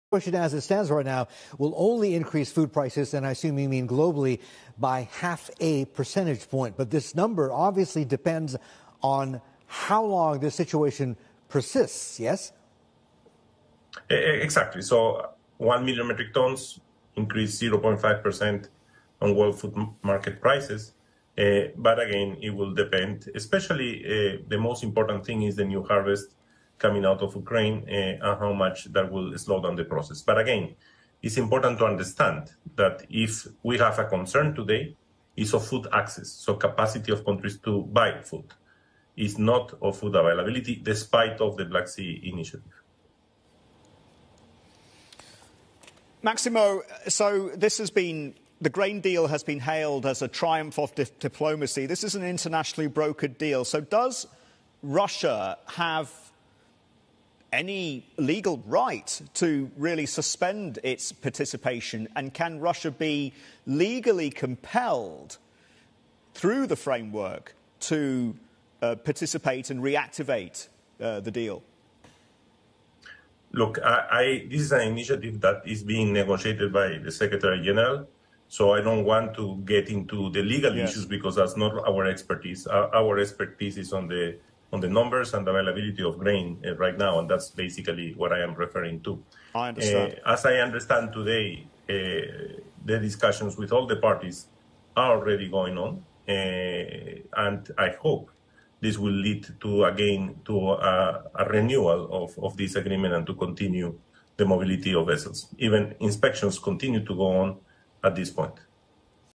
Russia's exit from grain deal could raise food prices: U.N.
Maximo Torero of the U.N. Food and Agriculture Organization discusses the consequences of Russia's exit from its grain deal with Ukraine.